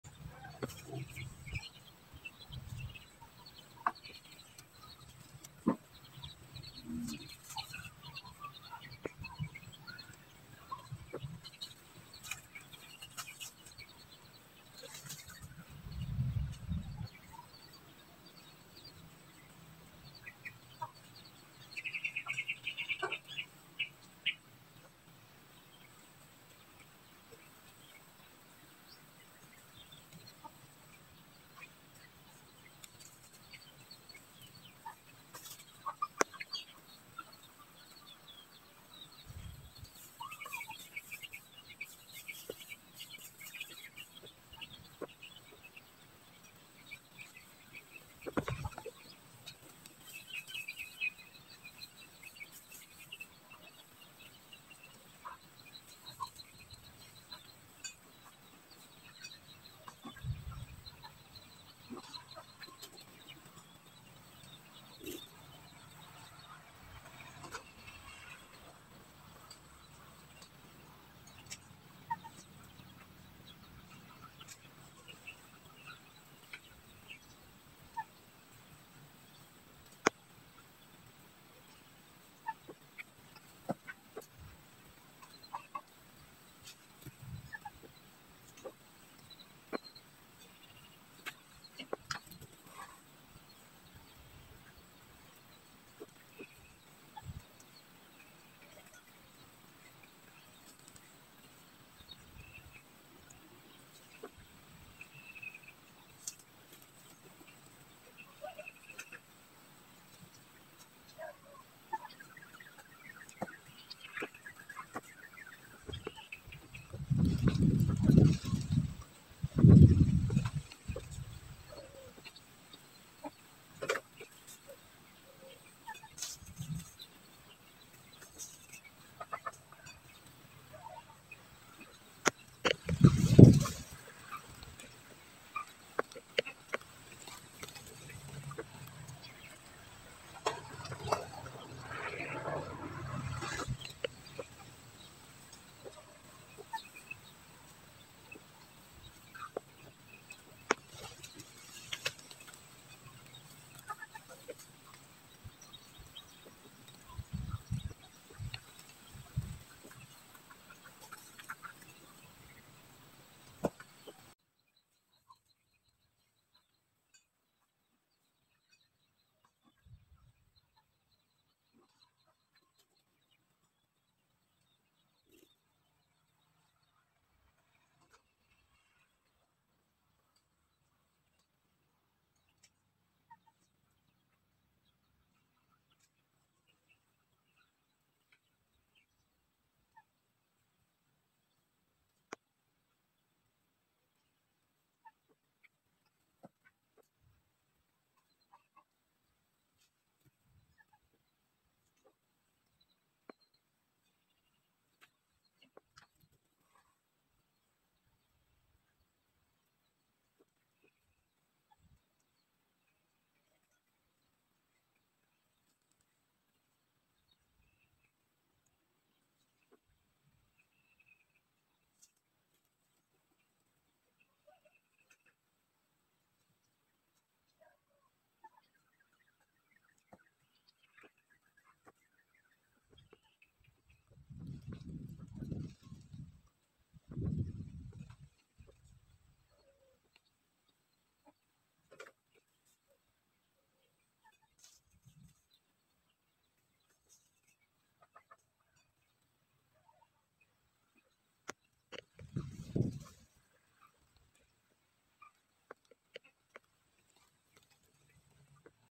Society finch is with the young that have just hatched. [ @My Nest nests ]
Hello! everyone wellcome to my channel youtube ( My Nest nests )